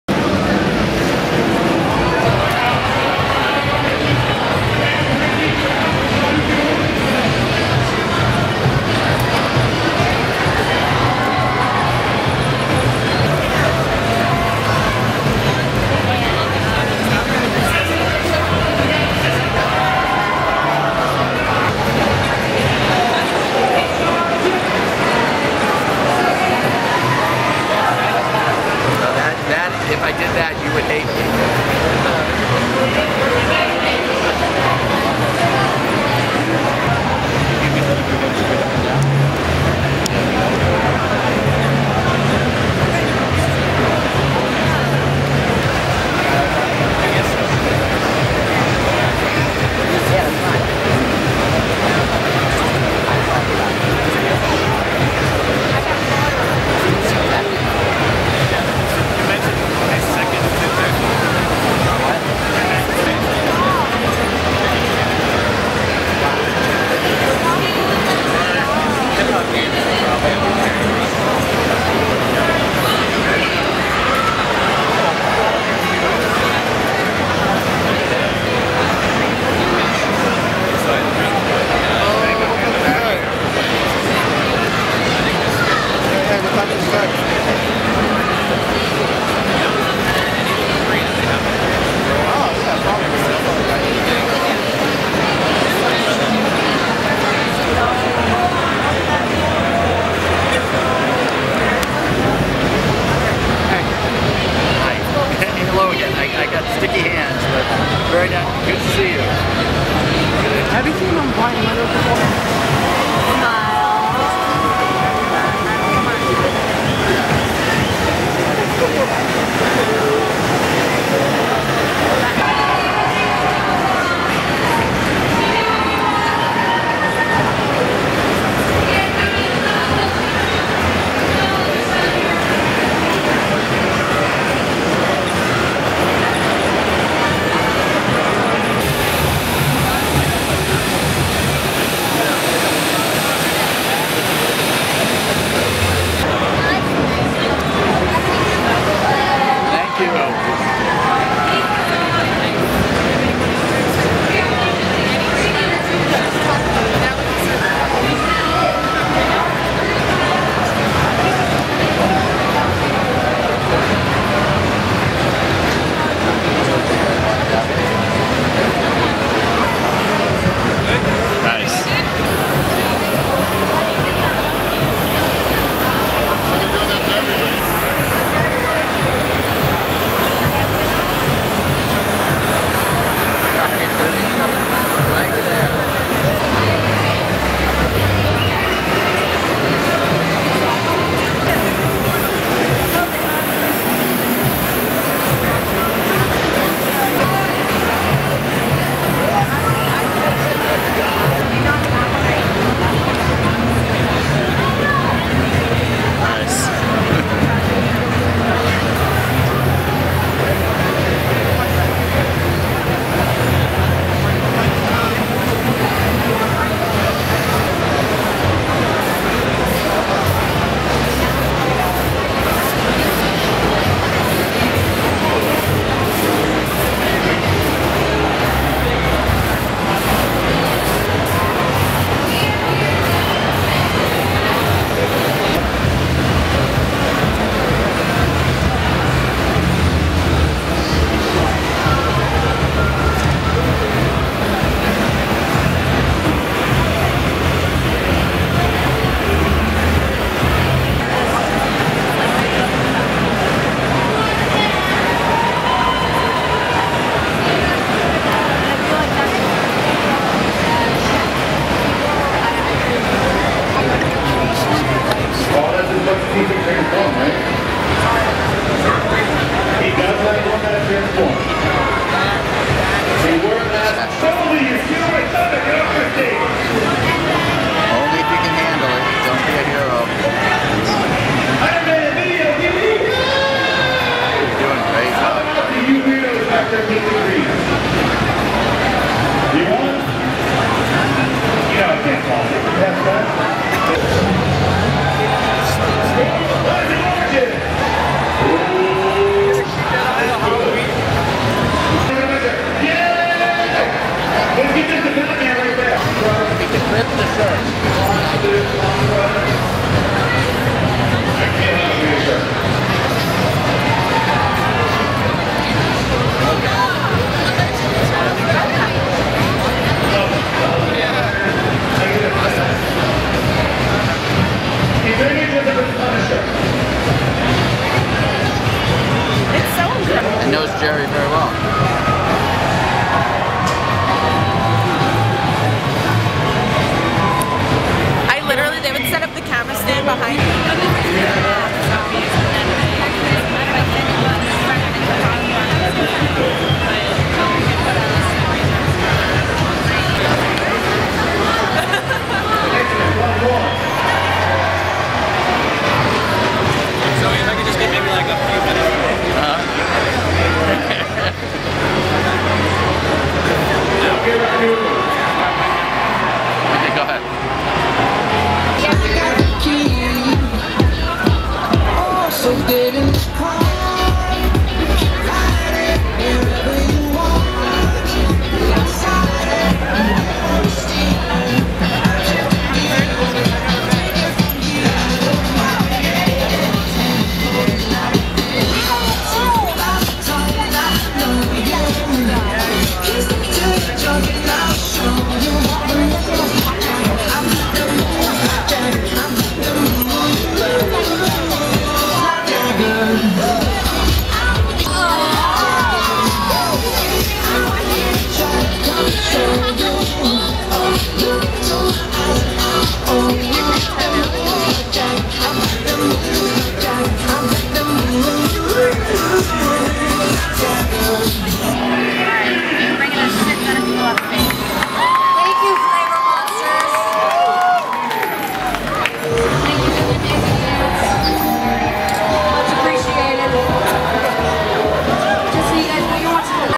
Flavor Monsters Transformation - NYCC 2012
Rod Maxwell Transforms truth® tour rider Edwin Contreras into a Strawberry Flavor Monster. Seven Hours of Work Compressed into a Seven Minute Video!